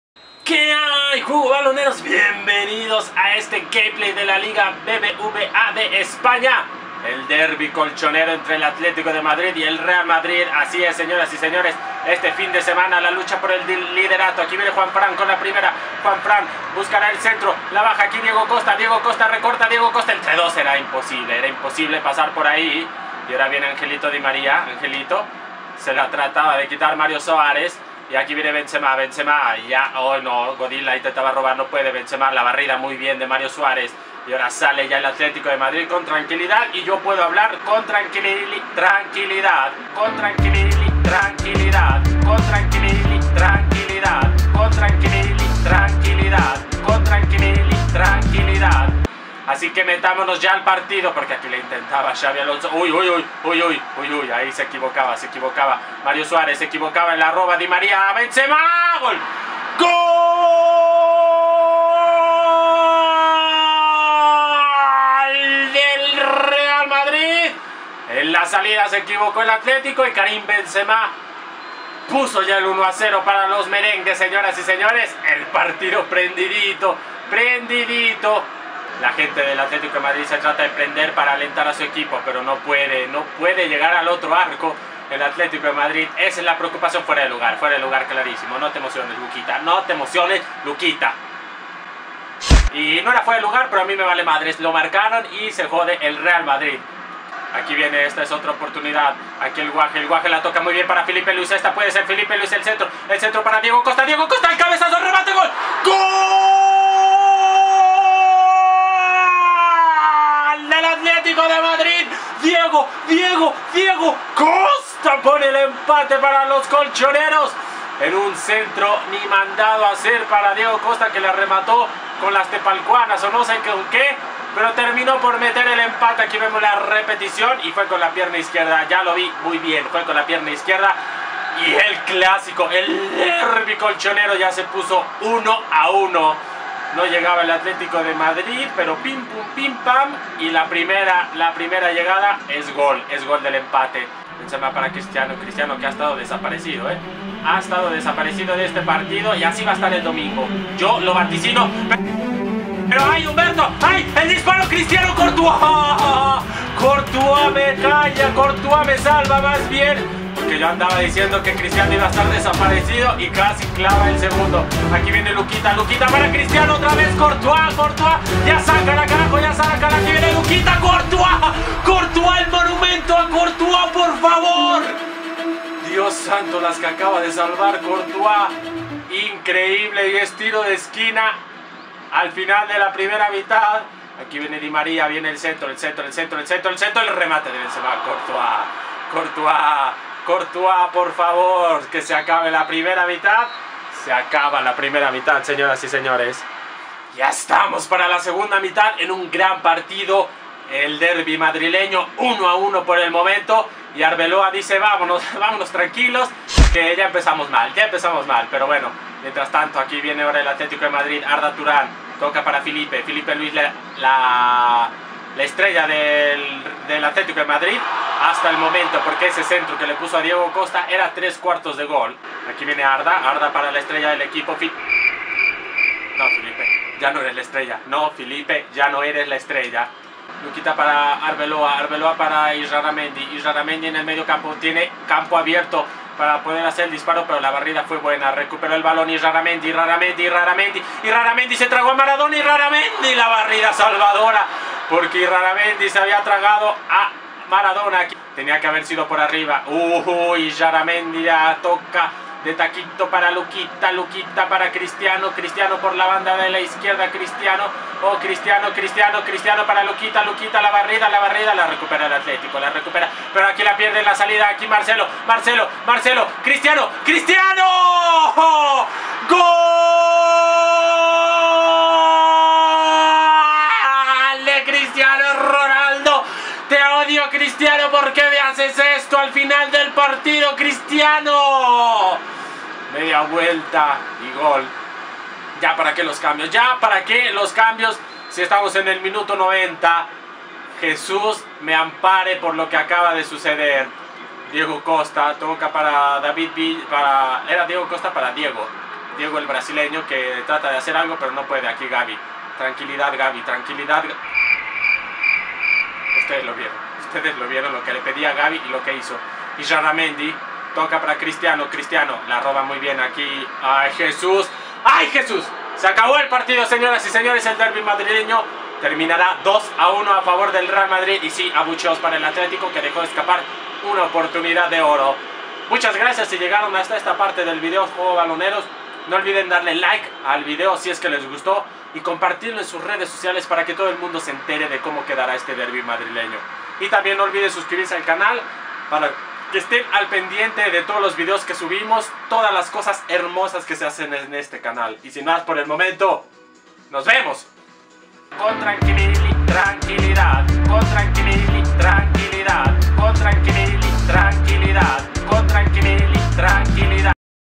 ATLÉTICO DE MADRID VS REAL MADRID | ¡¡Te odio Cristiano!!
El 'Derby Madrileño' traído a ustedes juegobaloneros, para que le apuesten el Domingo y ahora si juro que ganarán ;)